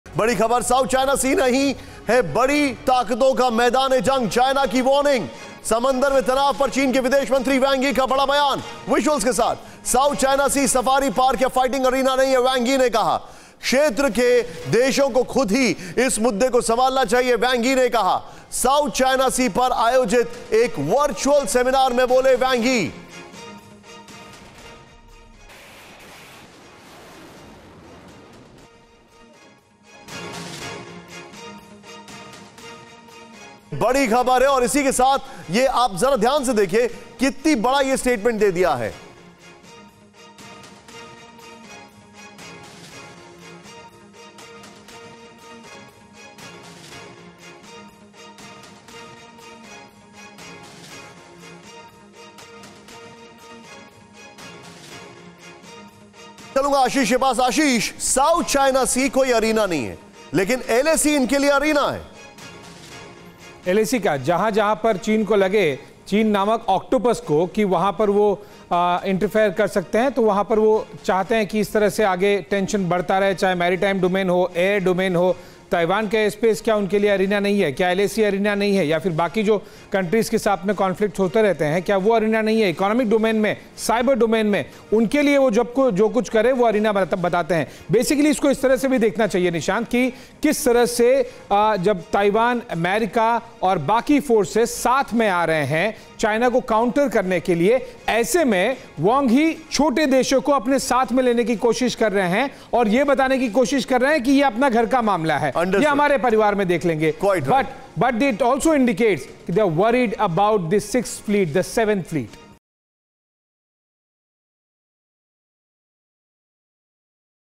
समंदर में तनाव पर चीनी विदेशमंत्री Wang Yi का बड़ा बयान, South China Sea नहीं है तनाव का केंद्र
साउथ चाइना सी नहीं है बड़ी ताकतों का मैदान-ए-जंग
समंदर में तनाव पर चीनी विदेशमंत्री वांग यी का बड़ा बयान
साउथ चाइना सी safari park या fighting arena नहीं- वांग यी
क्षेत्र के देशों को खुद ही इस मुद्दे को संभालना चाहिए- वांग यी
साउथ चाइना सी पर आयोजित एक वर्चुअल सेमिनार में बोले वांग यी

Credit